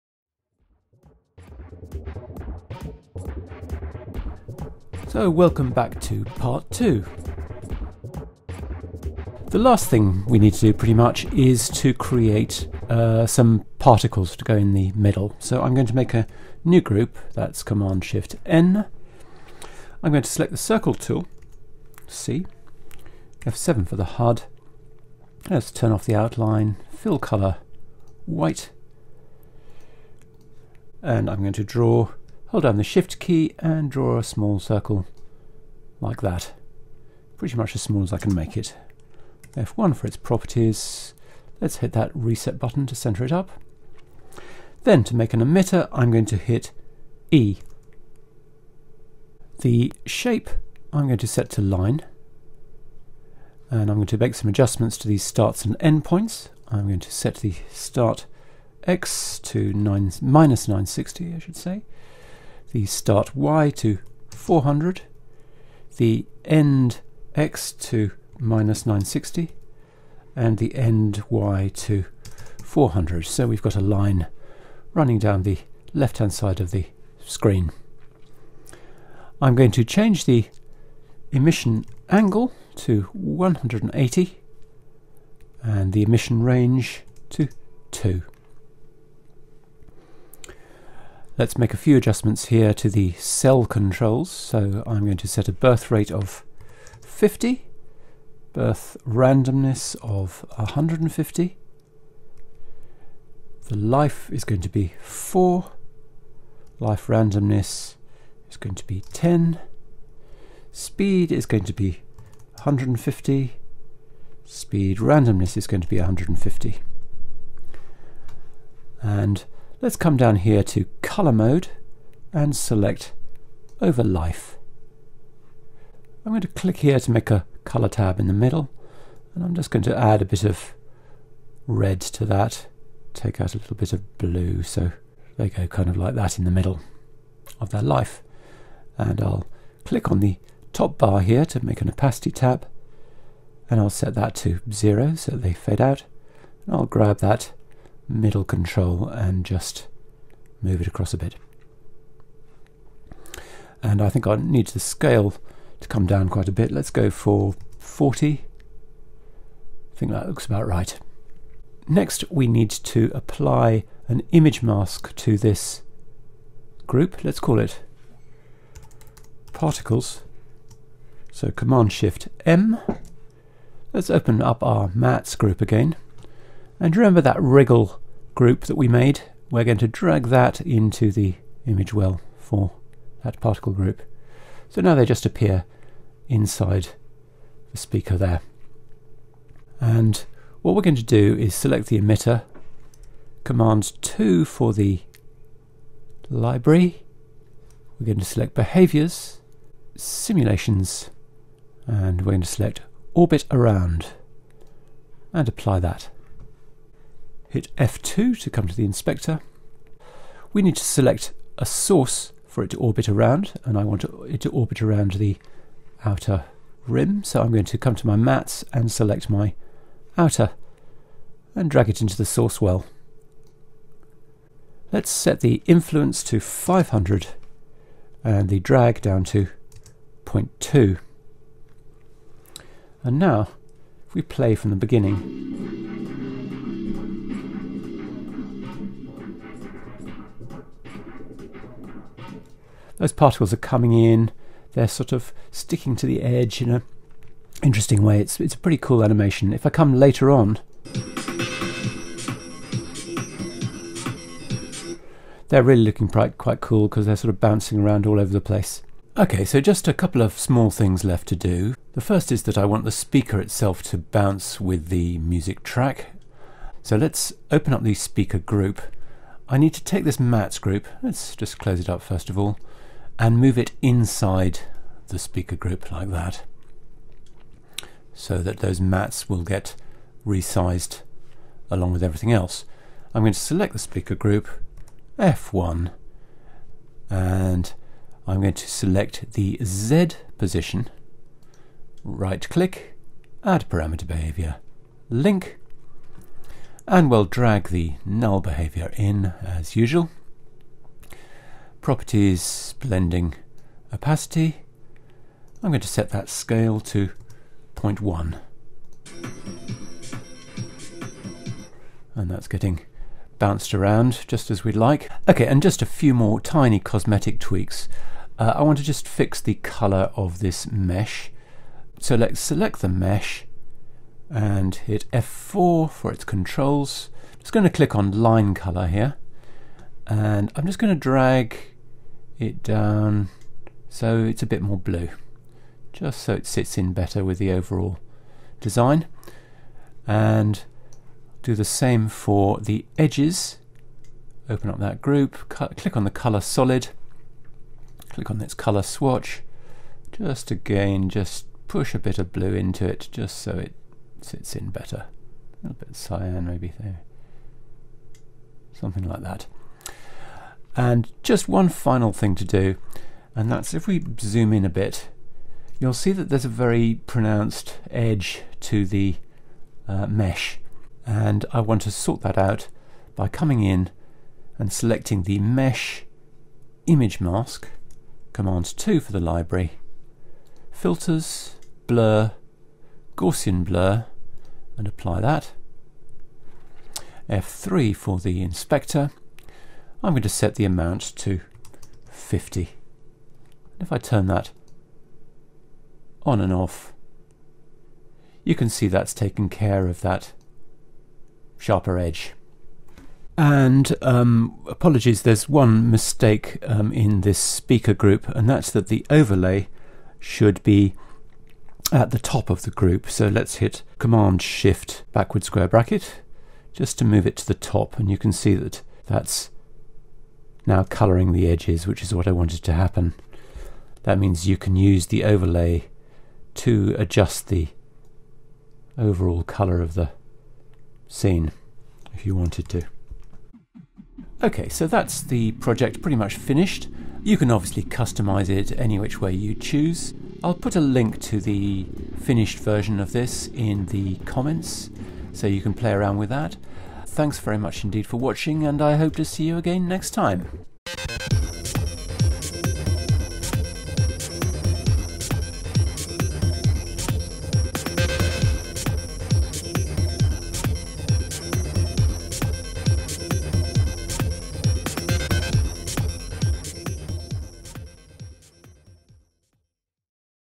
Apple Motion 5: NEW Visualizer Tutorial - Part 2
New Music Visualizer tutorial featuring a host of interesting techniques, useful even if music visualizers aren't your thing!